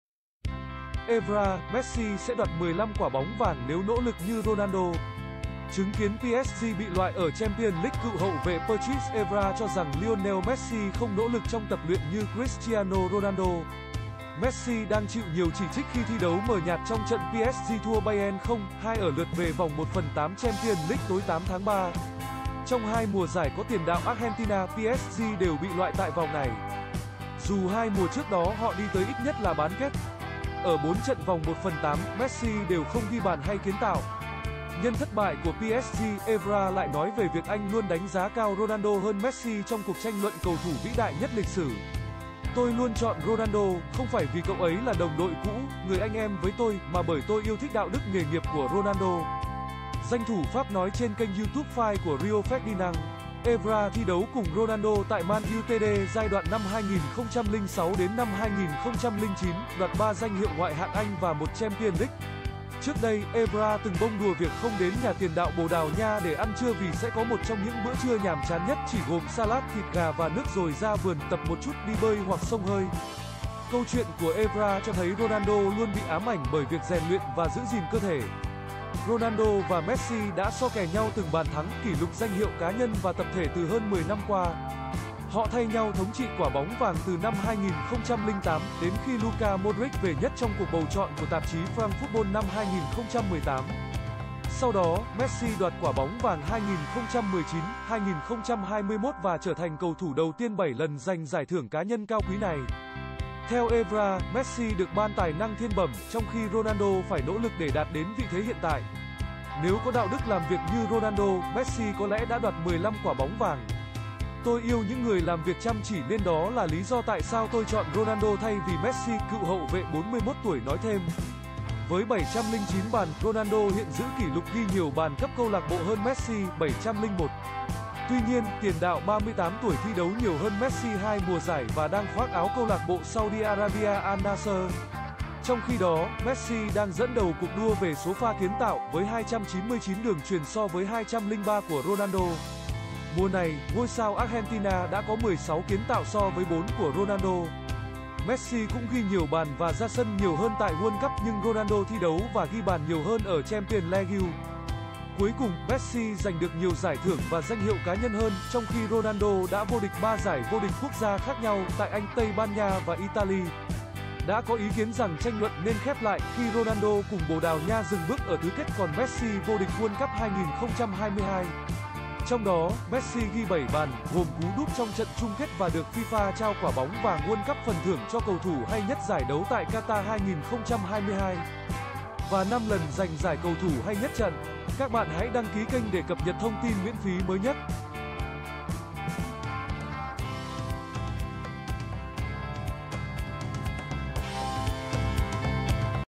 Evra: 'Messi sẽ đoạt 15 Quả Bóng Vàng nếu nỗ lực như Ronaldo'
Chứng kiến PSG bị loại ở Champions League, cựu hậu vệ Patrice Evra cho rằng Lionel Messi không nỗ lực trong tập luyện như Cristiano Ronaldo.

Messi đang chịu nhiều chỉ trích khi thi đấu mờ nhạt trong trận PSG thua Bayern 0-2 ở lượt về vòng 1/8 Champions League tối 8/3. Trong hai mùa giải có tiền đạo Argentina, PSG đều bị loại tại vòng này, dù hai mùa trước đó họ đi tới ít nhất là bán kết. Ở bốn trận vòng 1/8, Messi đều không ghi bàn hay kiến tạo.

Nhân thất bại của PSG, Evra lại nói về việc anh luôn đánh giá cao Ronaldo hơn Messi trong cuộc tranh luận cầu thủ vĩ đại nhất lịch sử. "Tôi luôn chọn Ronaldo, không phải vì cậu ấy là đồng đội cũ, người anh em với tôi, mà bởi tôi yêu thích đạo đức nghề nghiệp của Ronaldo", danh thủ Pháp nói trên kênh YouTube FIVE của Rio Ferdinand.

Evra thi đấu cùng Ronaldo tại Man Utd giai đoạn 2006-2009, đoạt ba danh hiệu Ngoại hạng Anh và một Champions League. Trước đây, Evra từng bông đùa việc không đến nhà tiền đạo Bồ Đào Nha để ăn trưa vì sẽ có một trong những bữa trưa nhàm chán nhất, chỉ gồm salad, thịt gà và nước rồi ra vườn, tập một chút, đi bơi hoặc xông hơi. Câu chuyện của Evra cho thấy Ronaldo luôn bị ám ảnh bởi việc rèn luyện và giữ gìn cơ thể.

Ronaldo và Messi đã so kè nhau từng bàn thắng, kỷ lục danh hiệu cá nhân và tập thể từ hơn 10 năm qua. Họ thay nhau thống trị Quả Bóng Vàng từ năm 2008, đến khi Luka Modric về nhất trong cuộc bầu chọn của tạp chí France Football năm 2018. Sau đó, Messi đoạt Quả Bóng Vàng 2019, 2021 và trở thành cầu thủ đầu tiên bảy lần giành giải thưởng cá nhân cao quý này.

Theo Evra, Messi được ban tài năng thiên bẩm, trong khi Ronaldo phải nỗ lực để đạt đến vị thế hiện tại. "Nếu có đạo đức làm việc như Ronaldo, Messi có lẽ đã đoạt 15 Quả Bóng Vàng. Tôi yêu những người làm việc chăm chỉ nên đó là lý do tại sao tôi chọn Ronaldo thay vì Messi", cựu hậu vệ 41 tuổi nói thêm.

Với 709 bàn, Ronaldo hiện giữ kỷ lục ghi nhiều bàn cấp CLB hơn Messi (701). Tuy nhiên, tiền đạo 38 tuổi thi đấu nhiều hơn Messi hai mùa giải và đang khoác áo CLB Saudi Arabia Al Nassr. Trong khi đó, Messi đang dẫn đầu cuộc đua về số pha kiến tạo, với 299 đường chuyền so với 203 của Ronaldo. Mùa này, ngôi sao Argentina đã có 16 kiến tạo so với bốn của Ronaldo.

Messi cũng ghi nhiều bàn và ra sân nhiều hơn tại World Cup, nhưng Ronaldo thi đấu và ghi bàn nhiều hơn ở Champions Laegue. Cuối cùng, Messi giành được nhiều giải thưởng và danh hiệu cá nhân hơn, trong khi Ronaldo đã vô địch ba giải VĐQG khác nhau, tại Anh, Tây Ban Nha và Italy.

Đã có ý kiến rằng tranh luận nên khép lại, khi Ronaldo cùng Bồ Đào Nha dừng bước ở tứ kết, còn Messi vô địch World Cup 2022. Trong đó, Messi ghi bảy bàn, gồm cú đúp trong trận chung kết và được FIFA trao Quả Bóng Vàng World Cup - phần thưởng cho cầu thủ hay nhất giải đấu tại Qatar 2022, và năm lần giành giải cầu thủ hay nhất trận.